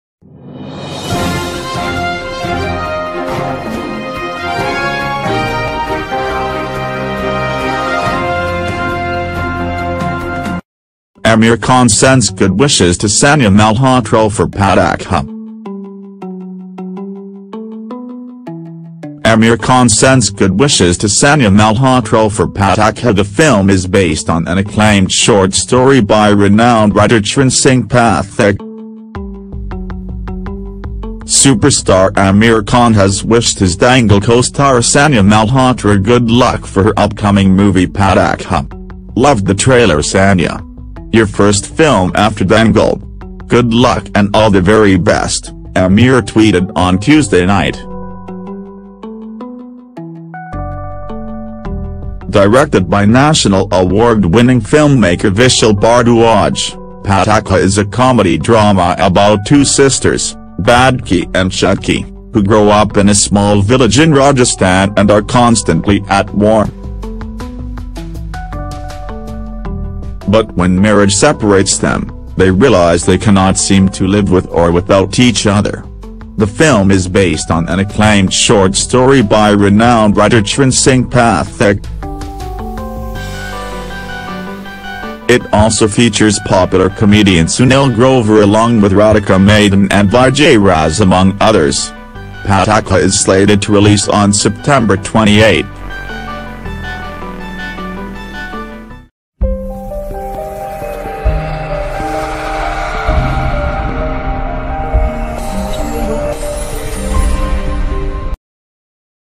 Aamir Khan sends good wishes to Sanya Malhotra for 'Pataakha'
The film is based on an acclaimed short story by renowned writer Charan Singh Pathik.